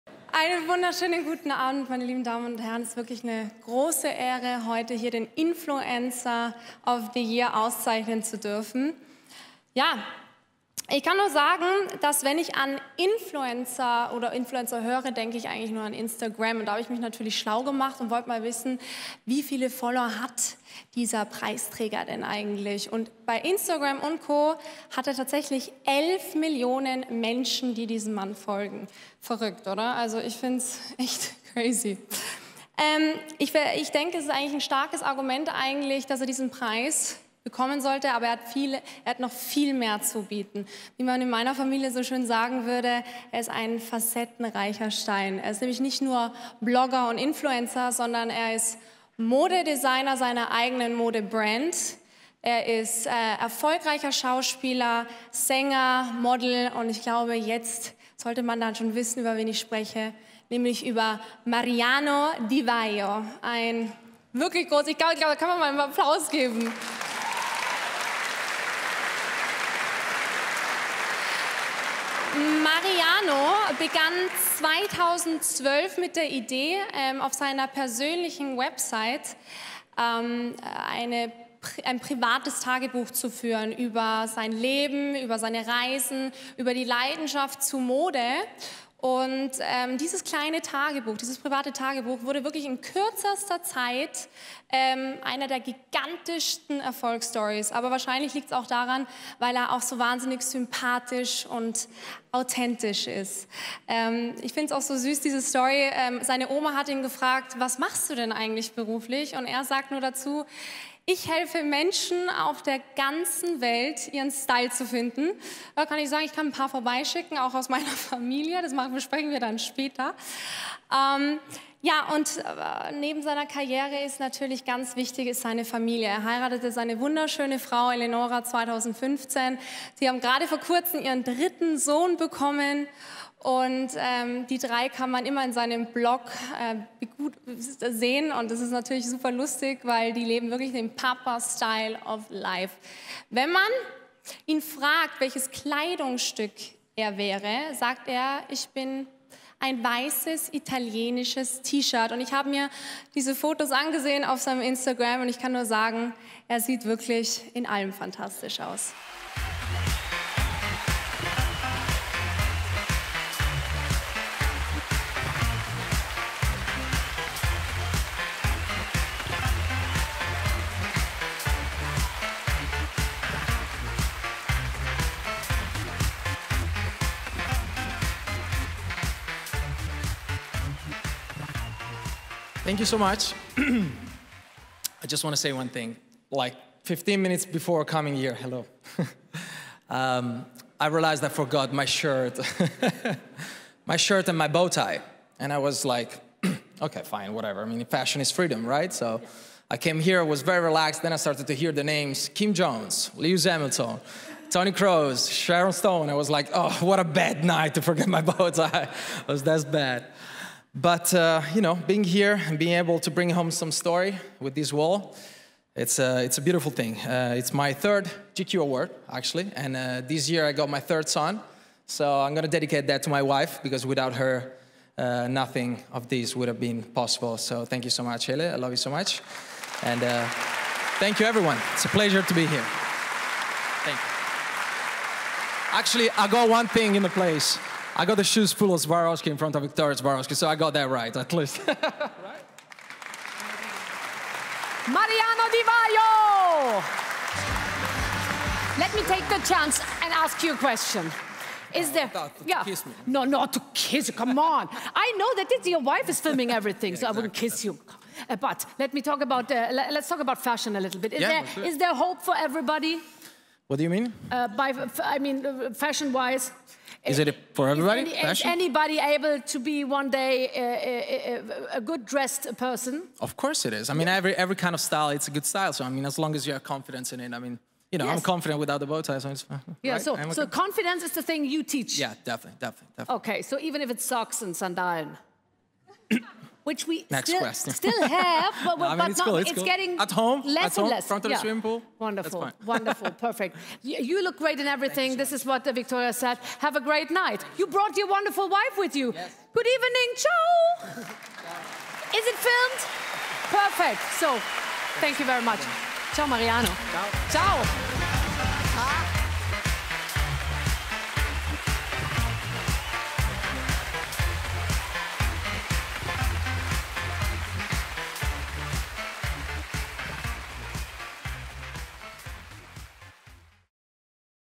Mariano Di Vaio gewinnt "Influencer of the Year"-Auszeichnung | GQ Men of the Year Awards 2019
Victoria Swarovski überreicht Mariano Di Vaio den GQ Award in der Kategorie "Influencer".